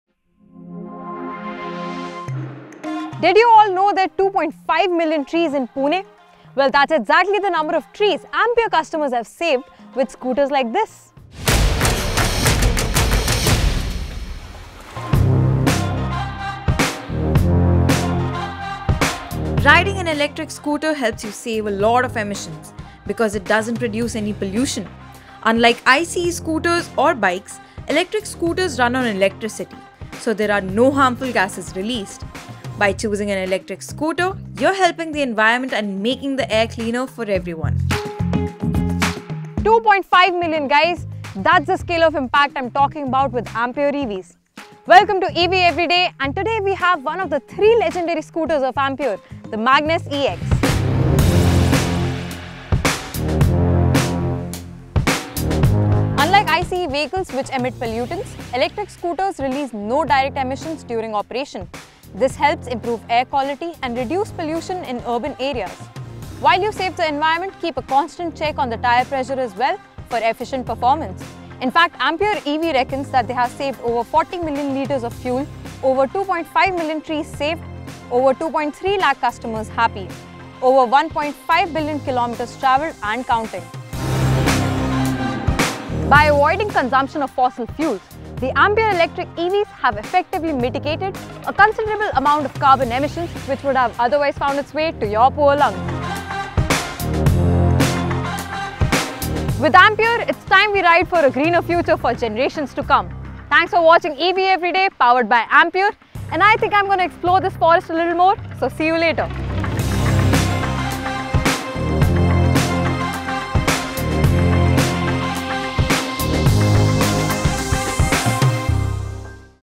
EV Everyday | How much do electric scooters reduce emissions? | PowerDrift
Dive into the eco-friendly world of electric scooters with 'EV Everyday'! In this episode, explore the remarkable reduction in emissions that electric scooters bring to our cities.